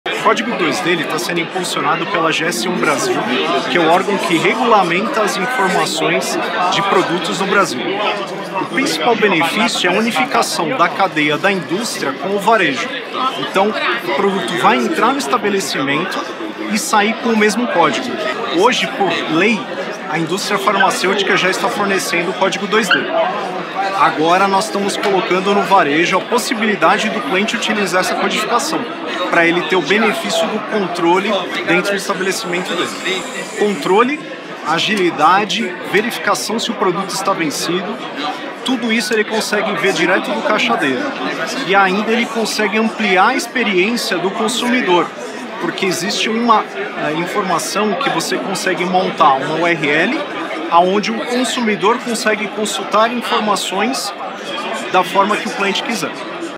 Prix 7T - Código 2D
✅ Lançamento Prix 7T

O Código 2D é uma tendência na rotulagem nutricional dos produtos, oferecendo mais informações e rastreabilidade aos consumidores. A Prix 7T conta com essa aplicação e para contar um pouco sobre os benefícios desse recurso, com a palavra o nosso Engenheiro de Soluções, Anderson Cetinic.

Uma balança inteligente, que traz um novo conceito ao mercado de pesagem.